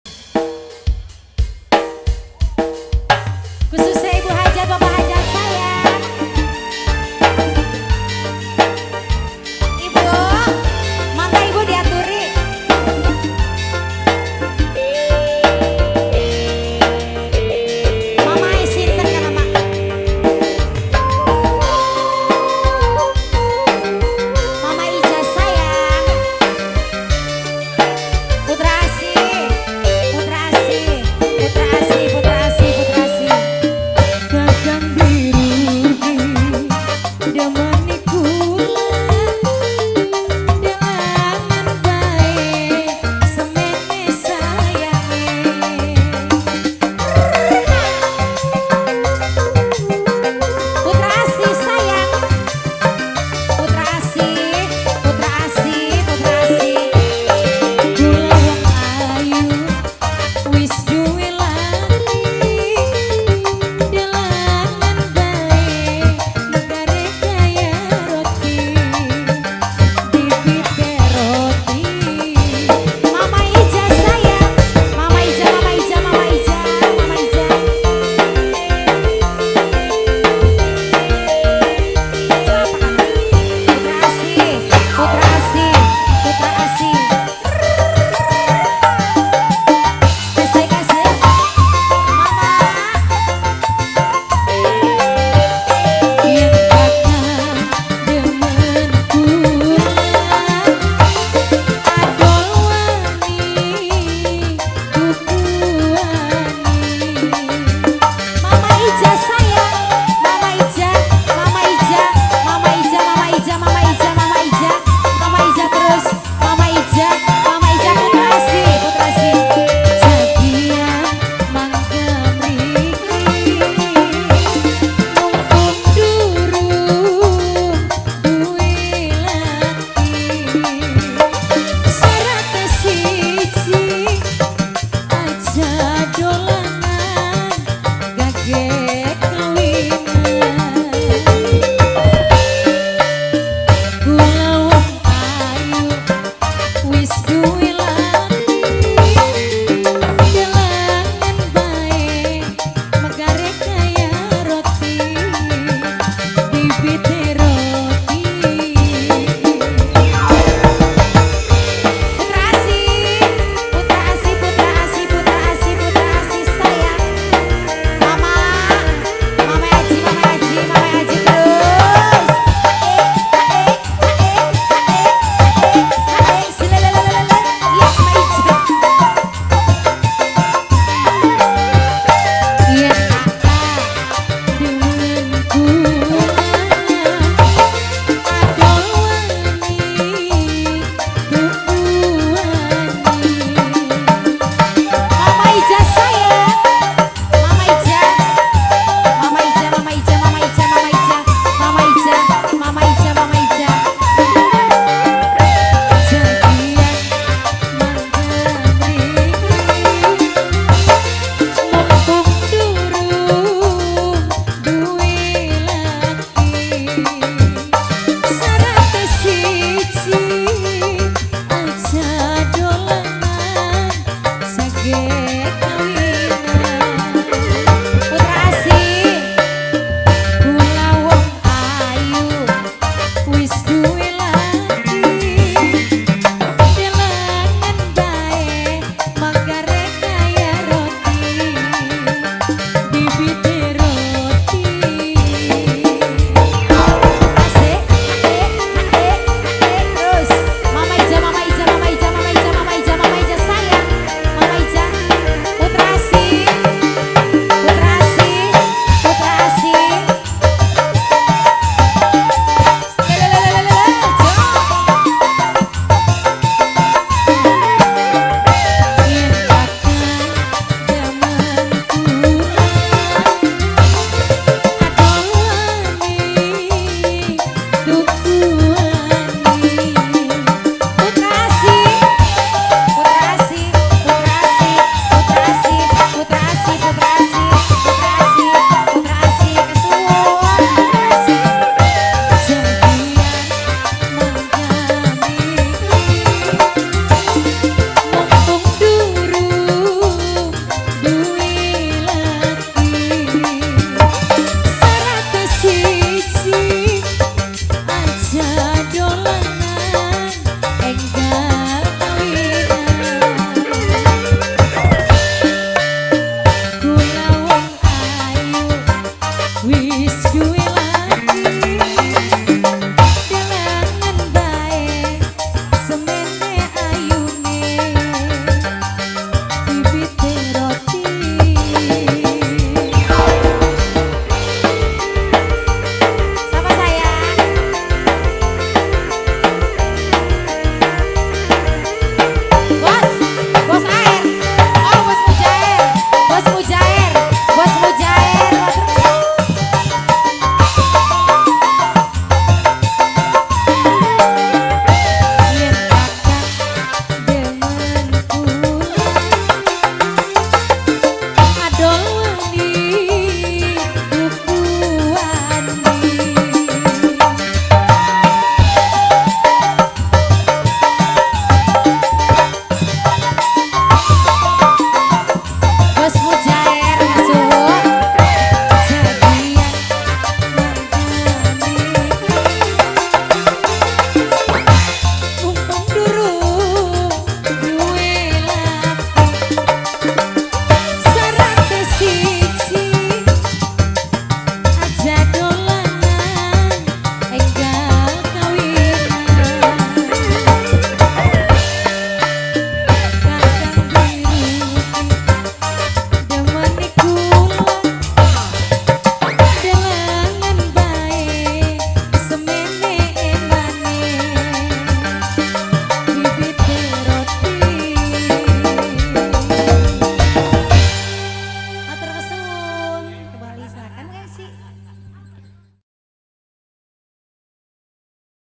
BIBIT ROTI DOKUMENTASI KHITANAN WAHYU SAPUTRA & RASULAN ARDELA ) LOMBANG 07 MEI 2023
YM PRODUCTION Video Shooting & Live Streaming ** Home Studio Blok Masjid Desa Lombang Kec. Juntinyuat - Indramayu ** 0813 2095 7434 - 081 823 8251